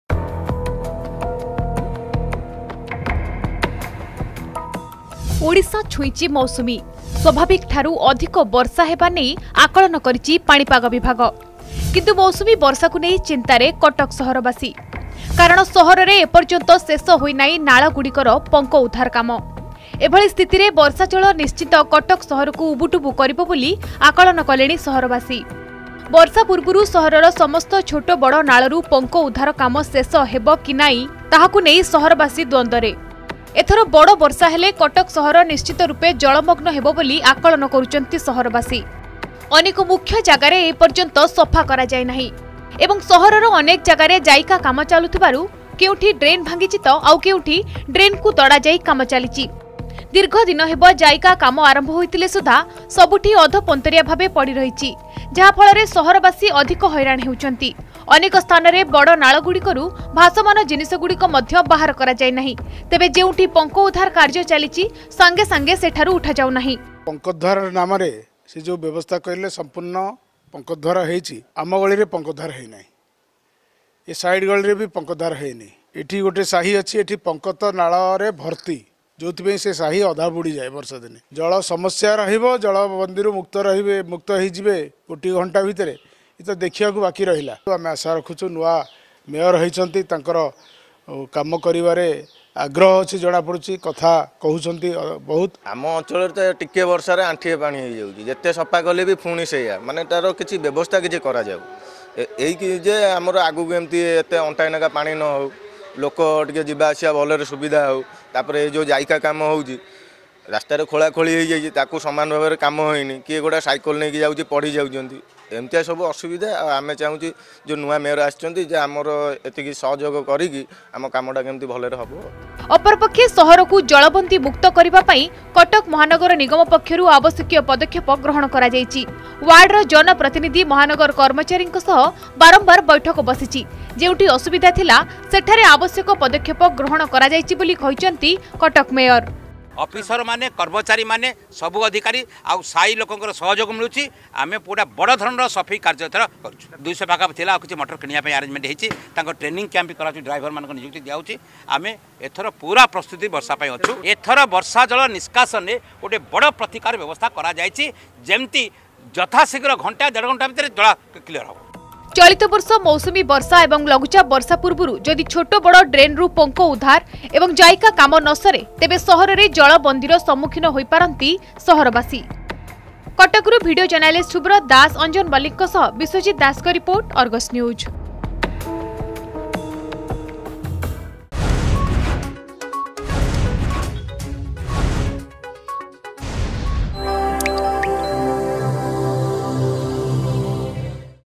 Monsoon reaches Odisha
Monsoon reaches Odisha, worried Cuttack people forecast about heavy rain fall